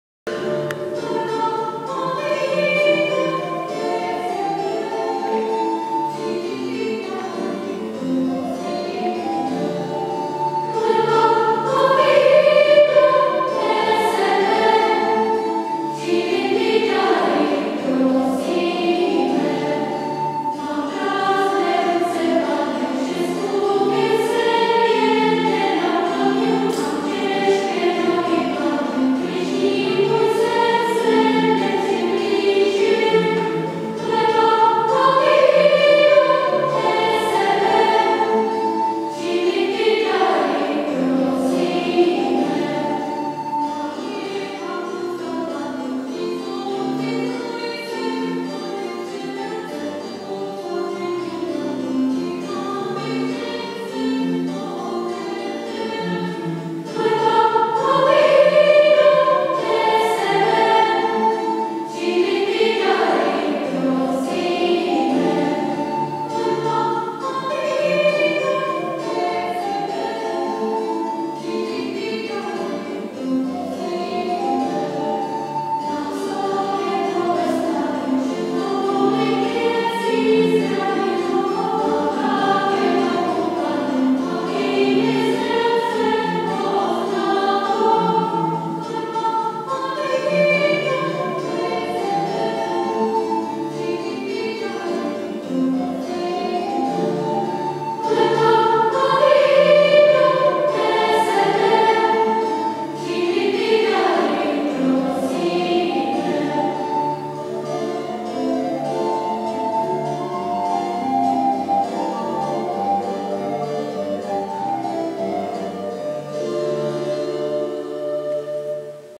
Sbor Radost_6
Koncert pražského sboru Radost v Neratovském kostele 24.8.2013